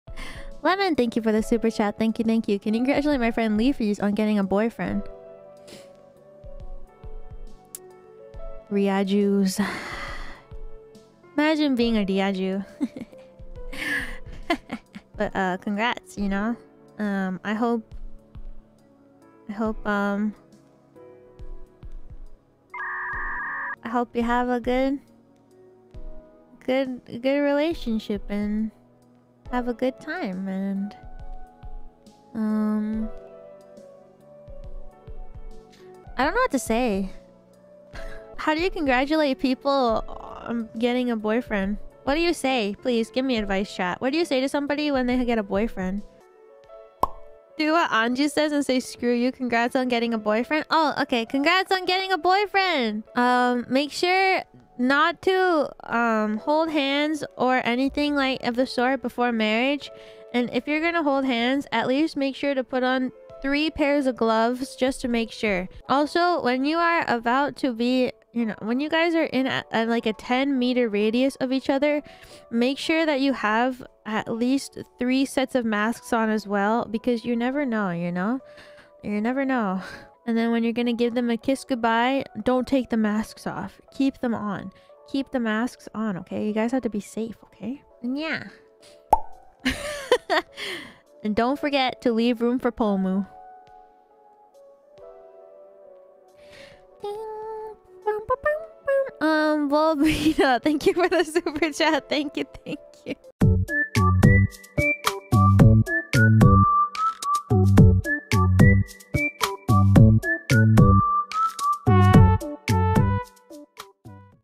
Elira gives Relationship Advice before Marriage 【NIJISANJI EN】
stay safe out there, fellow Fameliras!